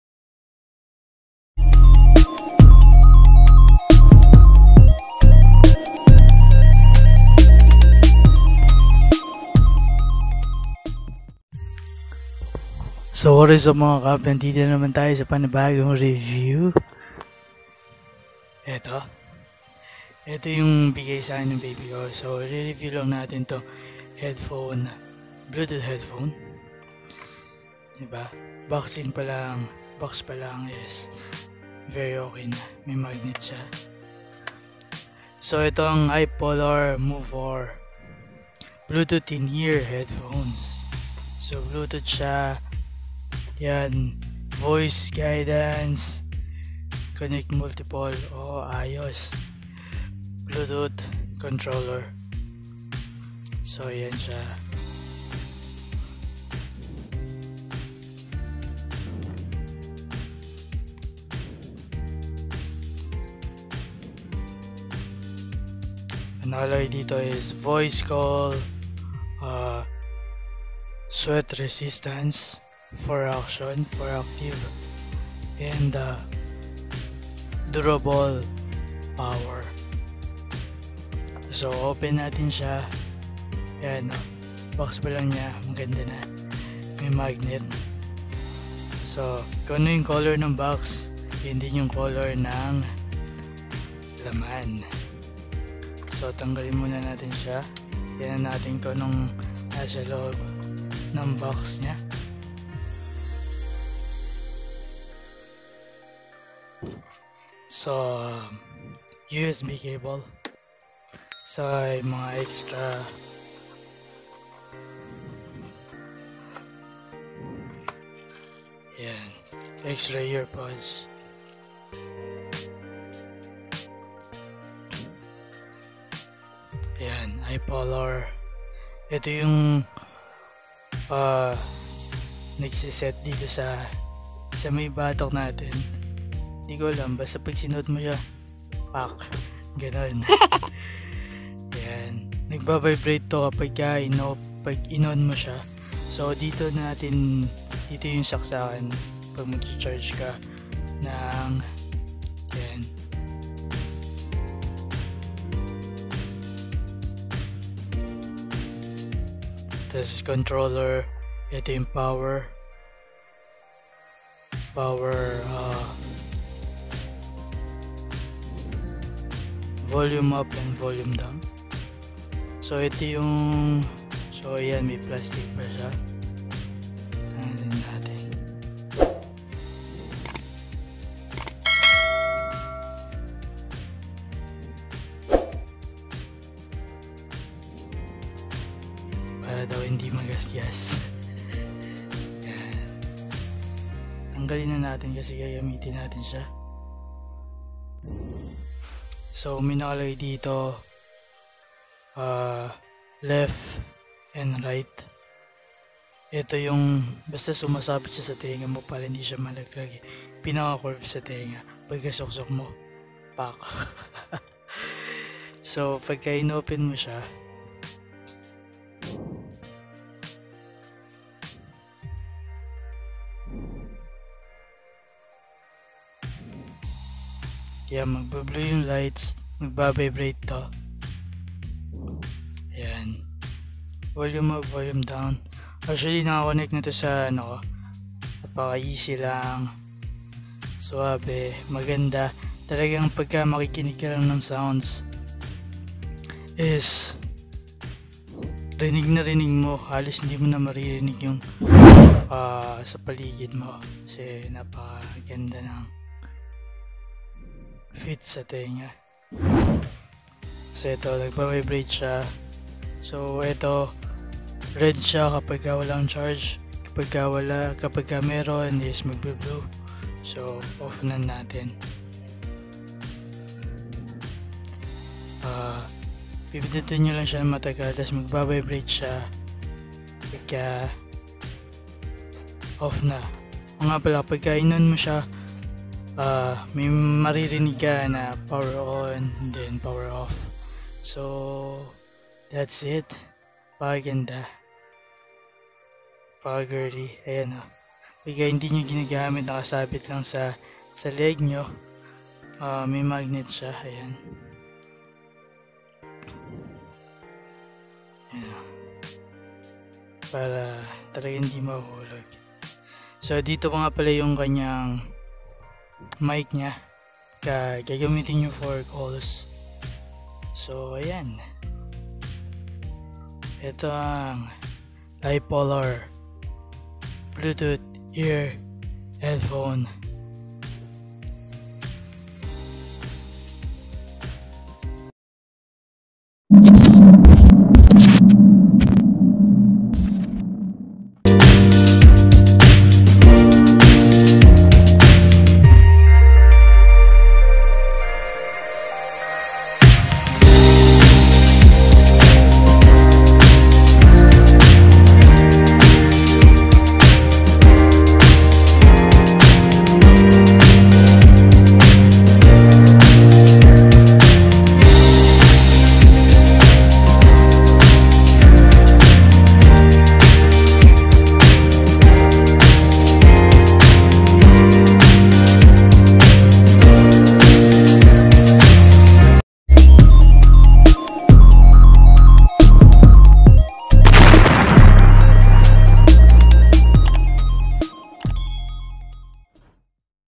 BLUETOOTH HEADPHONE REVIEW
Reviewinh this ipolar movear bluetooth headphone ( nit sponsored) , gift of my babe